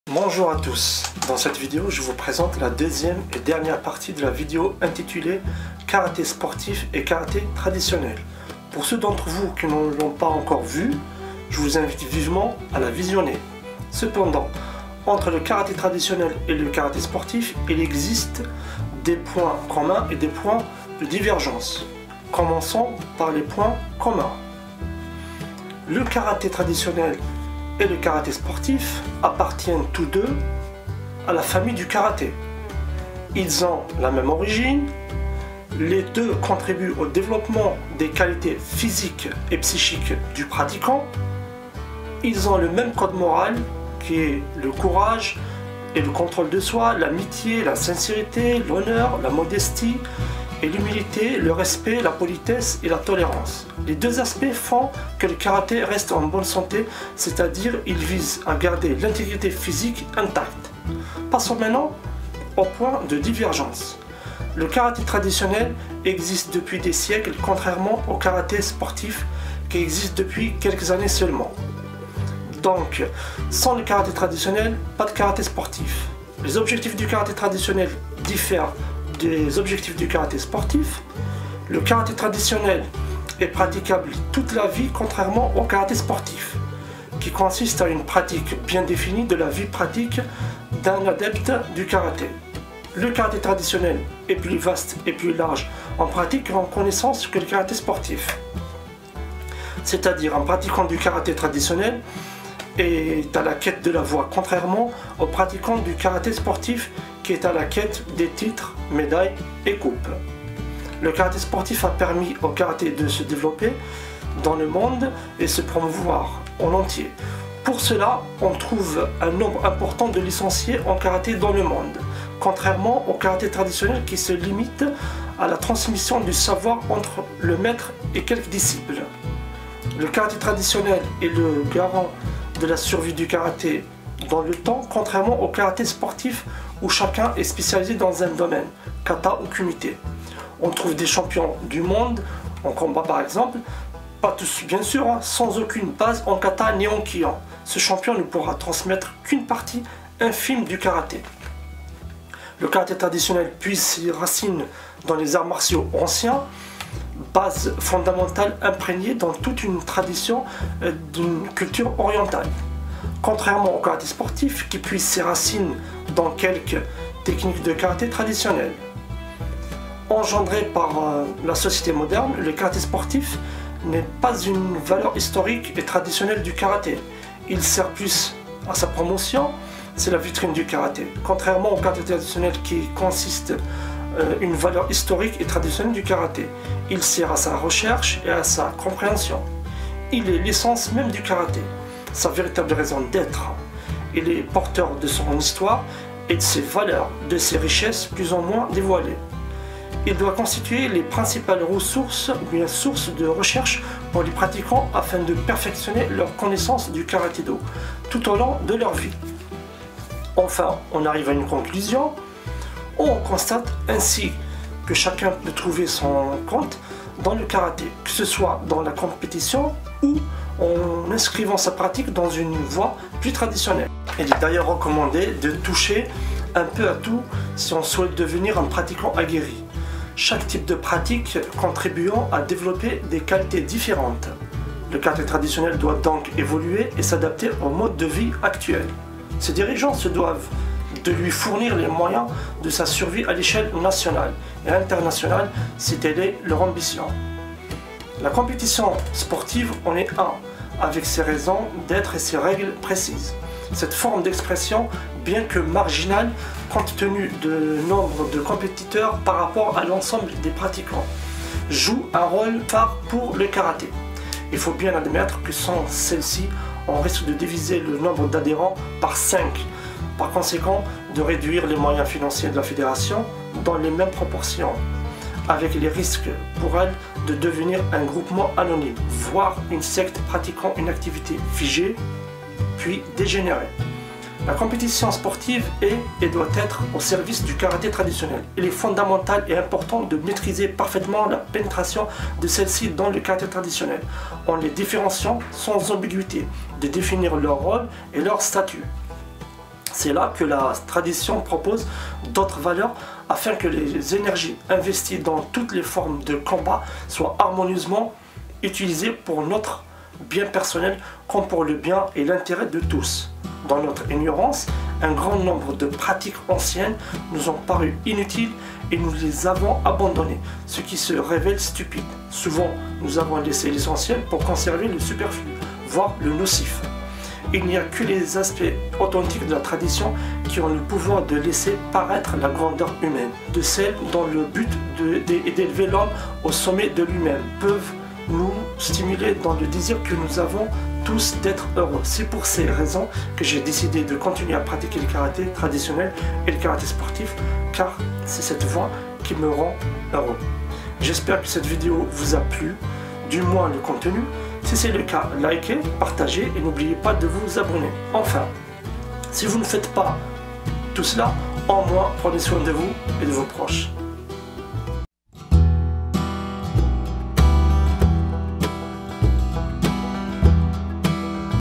LE KARATÉ TRADITIONNEL ET LE KARATÉ SPORTIF #2
Dans cette vidéo, je vous présente la deuxième partie du karaté traditionnel et du karaté sportif afin de faire la différence entre les deux pour pouvoir suivre l'un ou l'autre voir même les deux dans votre pratique.